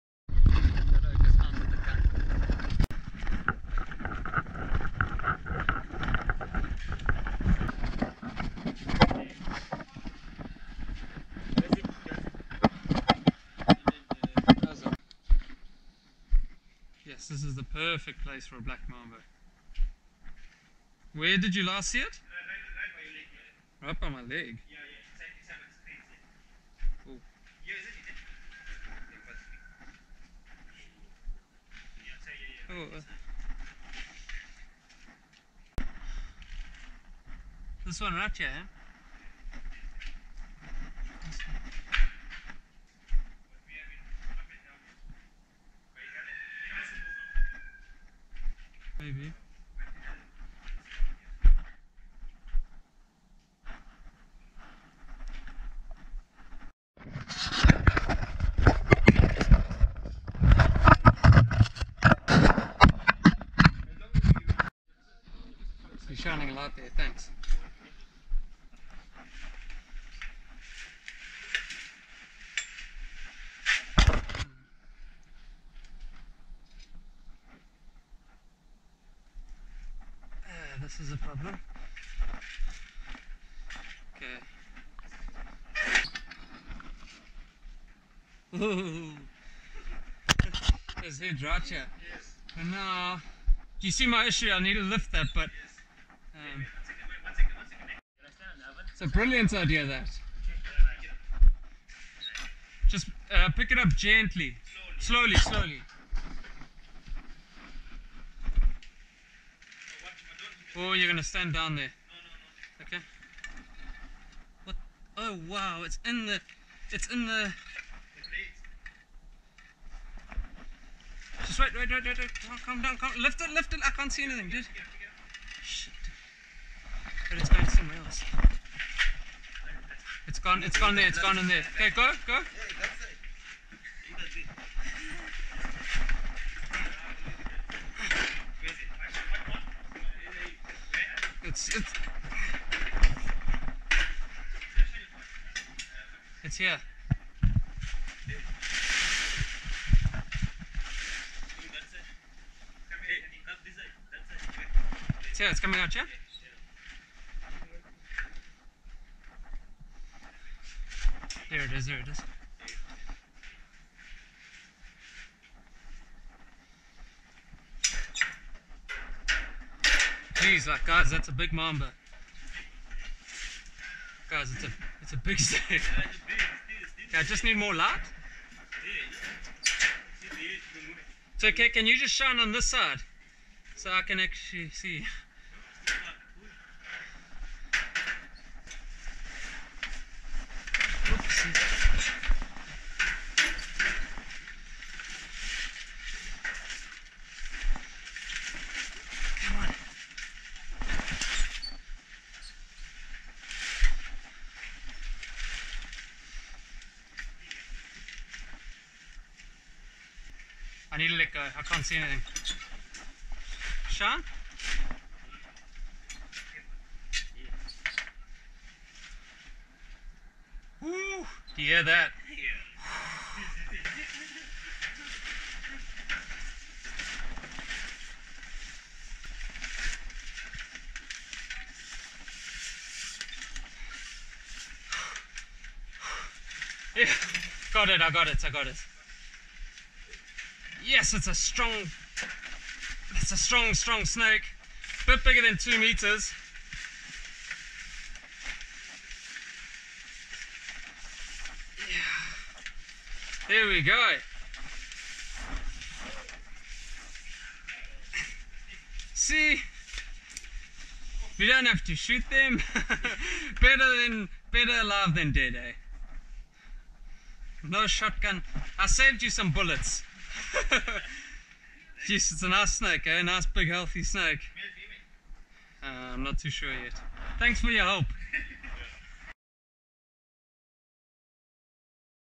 BLACK MAMBA IN A SCRAPYARD!
This was a super exciting yet challenging rescue call, but one of the most worthwhile rescues I've been on in ages!

This family previously used to kill snakes on site, until one person in the family started following me on social media. So this time, they called for help, and thank goodness they did!
Their whole attitude had changed after this. Really special people, and I'm so glad I could help them, and this beautiful snake!